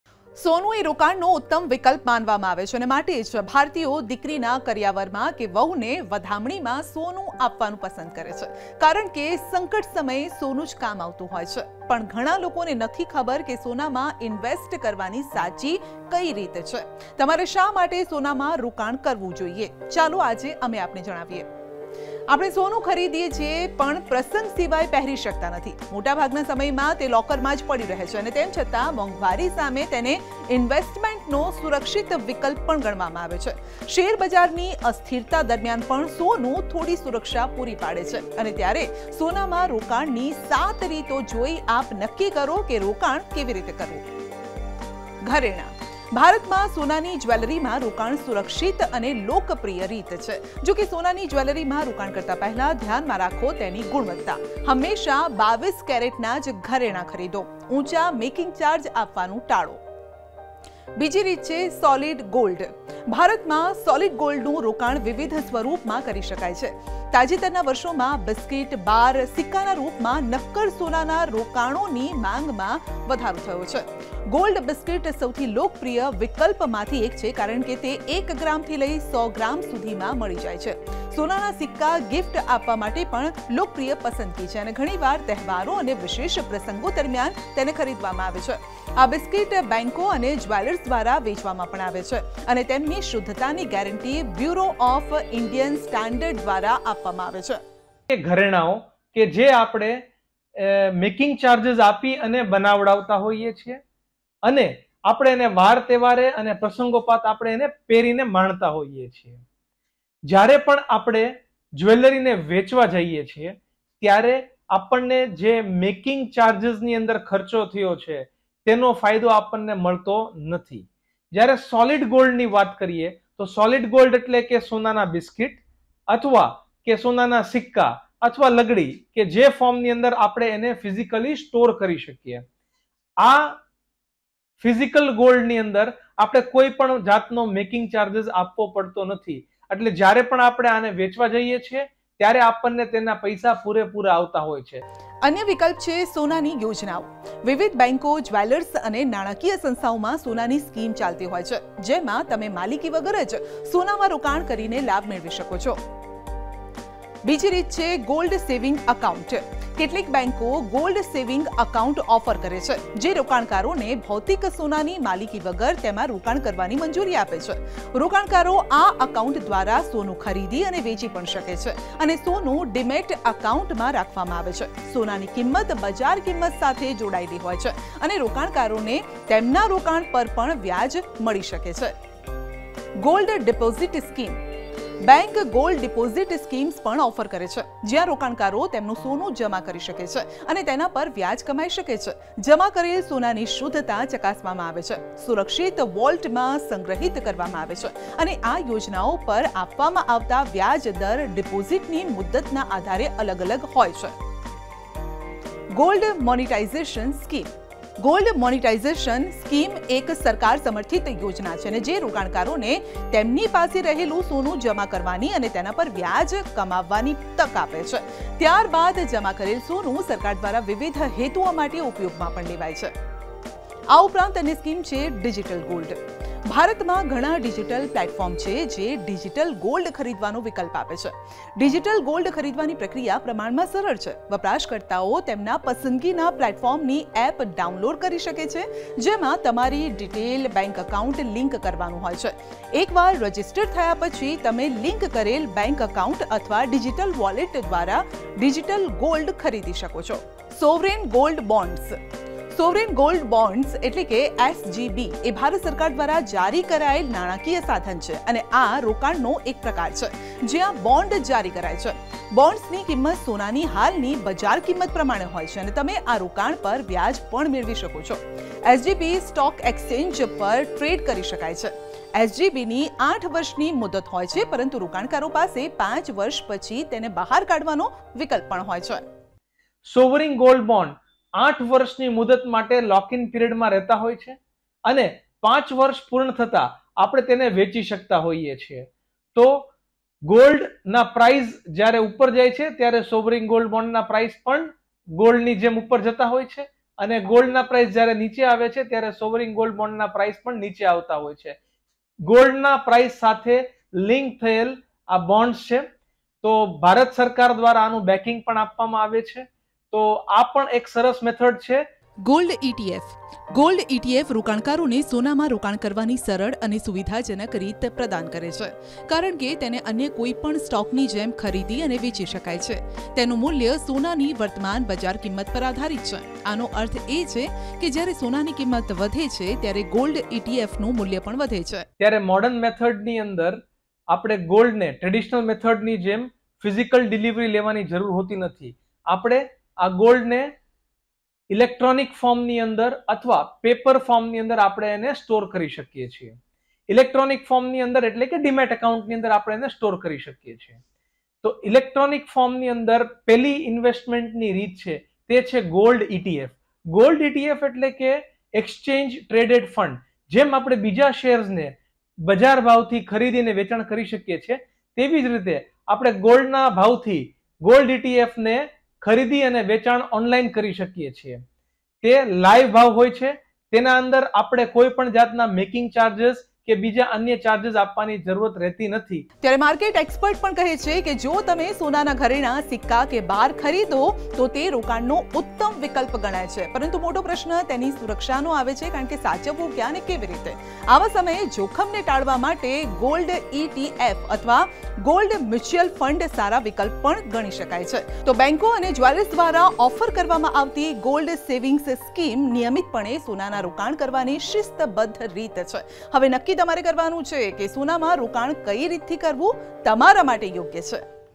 Gold । સોનામાં રોકાણ કરતાં પહેલા જુઓ આ વીડિયો ! | Gold Buy । Gujarati News । News 18 Gujarati । N18V
We buy gold but can't wear it except on occasion.most of the time it stays in the locker.yet it is considered as a safe investment option against inflation.even during stock market volatility gold provides some security, while gold Decide how to invest by looking at 7 ways to invest

આપણે સોનું ખરીદીએ છીએ પણ પ્રસંગ સિવાય પહેરી શકતા નથી.મોટાભાગના સમયમાં તે લોકરમાંજ પડ્યું રહે છે.તેમછતાં મોંઘવારી સામે તેને ઈન્વેસ્ટમેન્ટનો સુરક્ષિત વિકલ્પ ગણવામાં આવે છે. શેરબજારની અસ્થિરતા દરમિયાન પણ સોનું થોડી સુરક્ષા પુરી પાડે છે, ત્યારે સોનામાં રોકાણની 7 રીતો જોઈ આપ નક્કી કરો કે રોકાણ કેવી રીતે કરવું.n18oc_gold